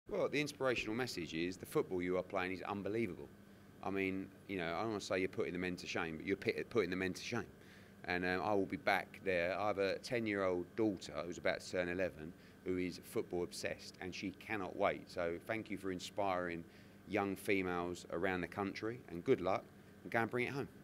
Eddie Hearn sends INSPIRATIONAL MESSAGE OF SUPPORT to THE LIONESSES!
Eddie Hearn sends his support to the Women's England Football Team ahead of their clash with Spain.